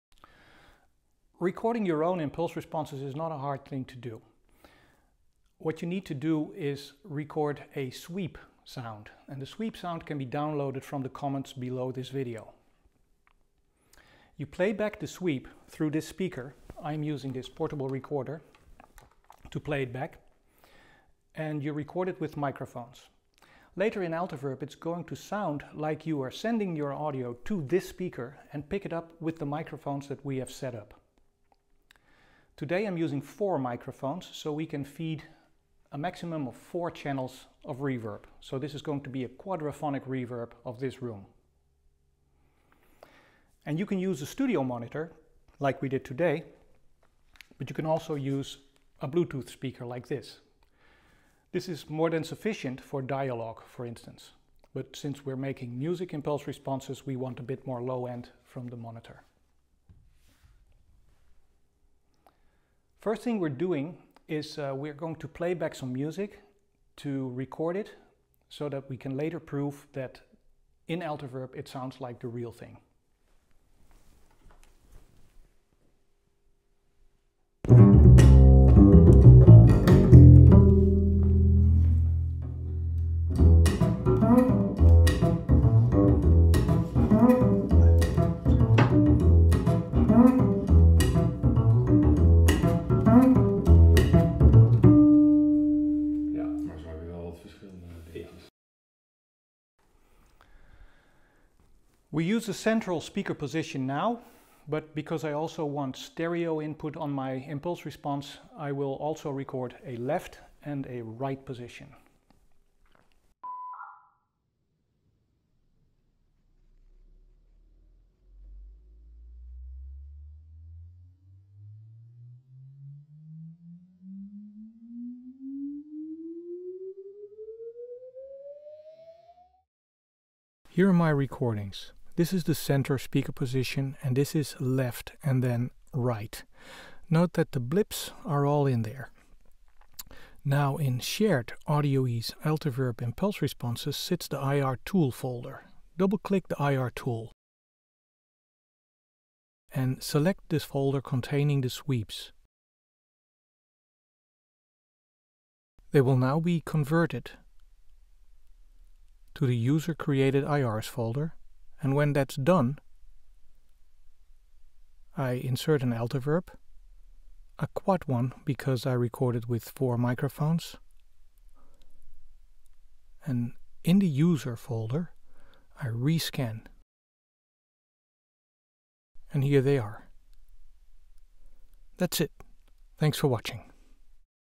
Making your own Altiverb Impulse Responses
To construct the shown spider microphone stand: at Thomann, Roadworx Thread Cube or Manfrotto Dado Kit, Roadworx Spacer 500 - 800 and Roadworx Thread Adapter 4

More tips and tricks in the Altiverb 8 Manual, on the last